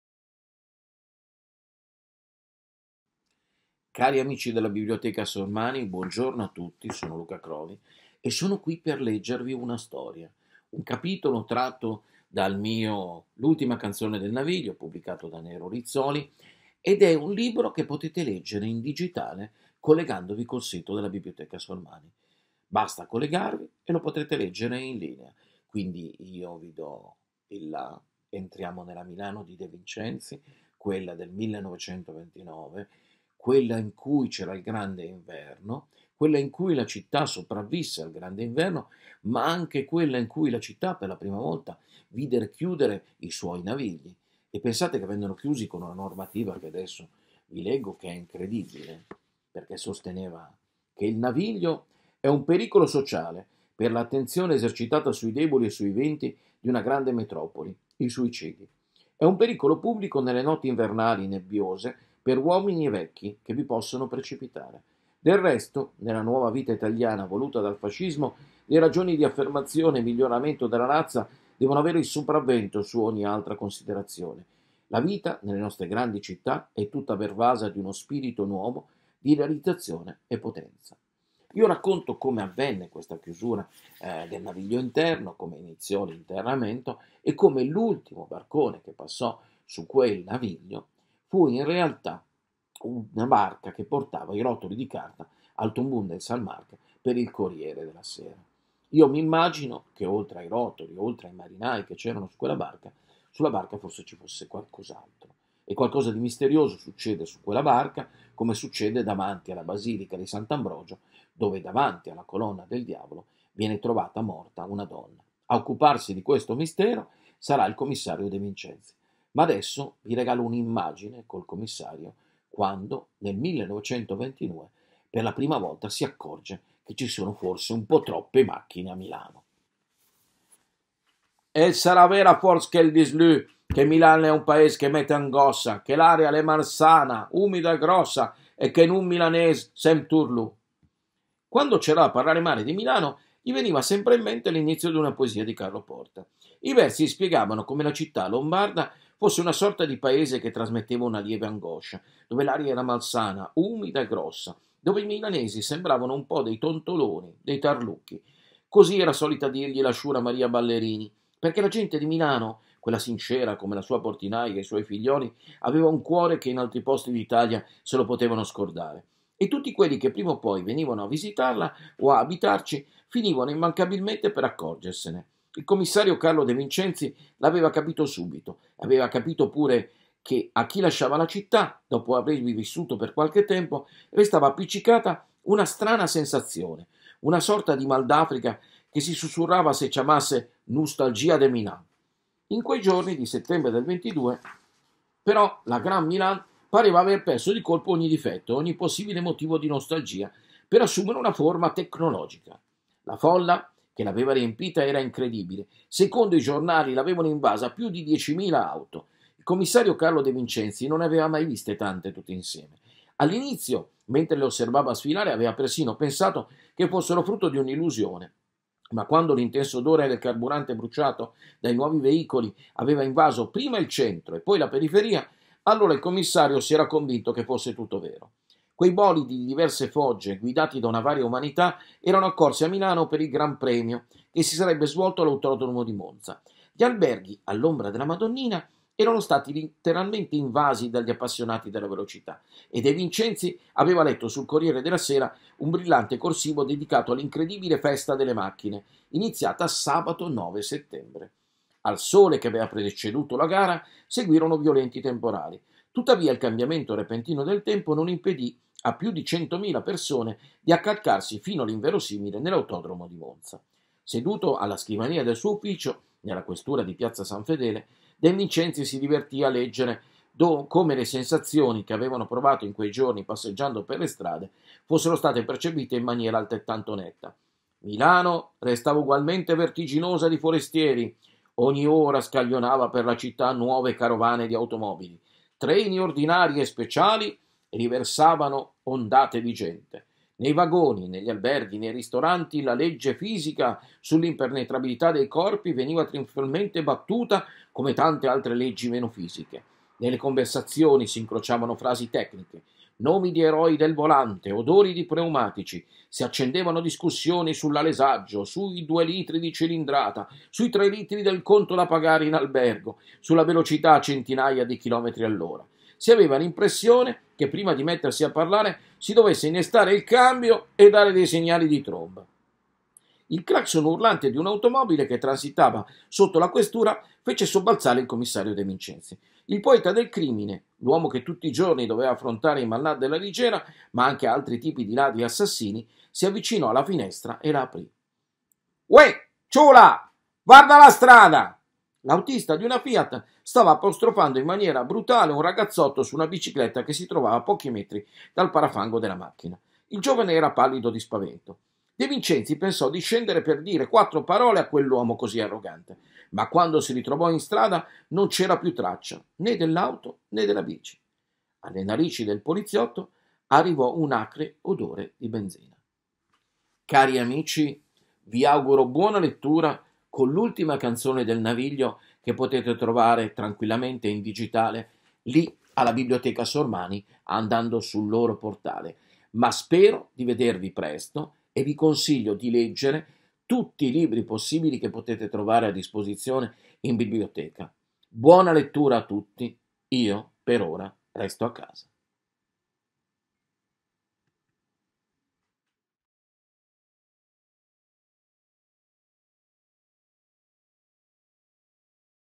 L'ultima canzone del Naviglio, di Luca Crovi
Luca Crovi legge un capitolo dal suo romanzo "L'ultima canzone del Naviglio" (2020)